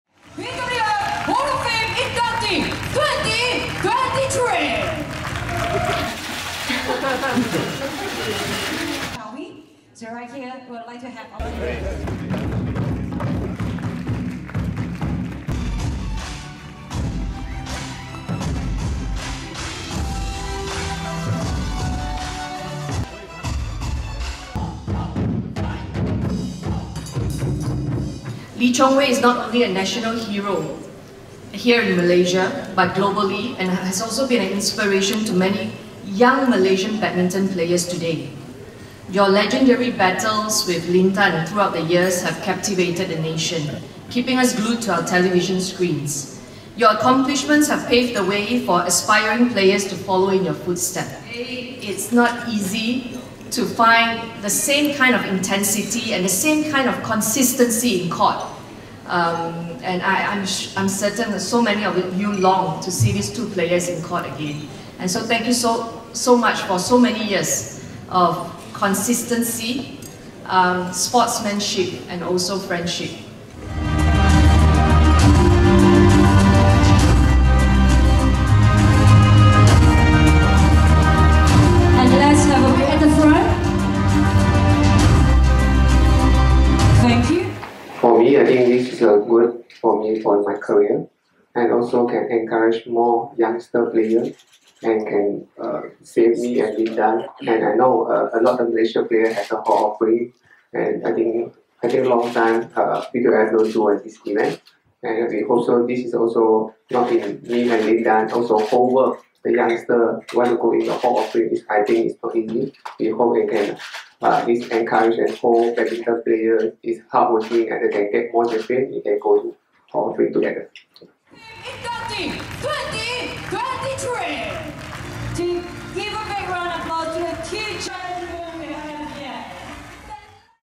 Badminton | BWF Hall Of Fame | Lee Chong Wei - Lin Dan
Two of badminton’s biggest stars of their generation, Lee Chong Wei and Lin Dan, received the ultimate honour of the sport on Friday (26 May) as they were inducted into the Badminton World Federation (BWF) Hall of Fame at the Kuala Lumpur Convention Centre in Malaysia.

It's another title they can add to their already impressive list of accolades.

Lee has collected 69 career titles and holds the record for most weeks spent as the world number one: 349 weeks. The 40-year-old shuttler is also the most decorated Malaysian Olympic athlete with his three silver medals.

“Being in the Hall of Fame is good for me and my career. I hope it can encourage more young players and show them that if they work hard, they can also be elected for the Hall of Fame,” Lee said at Friday’s press conference.

Lin of People’s Republic of China is a two-time Olympic champion and five-time world champion.

Throughout his 20 year-long career, Lee won 66 tournaments, including the All England Open six times.

“I feel very, very proud to be inducted to the Hall of Fame. I am very lucky with all I have achieved,” Lin said during the press conference.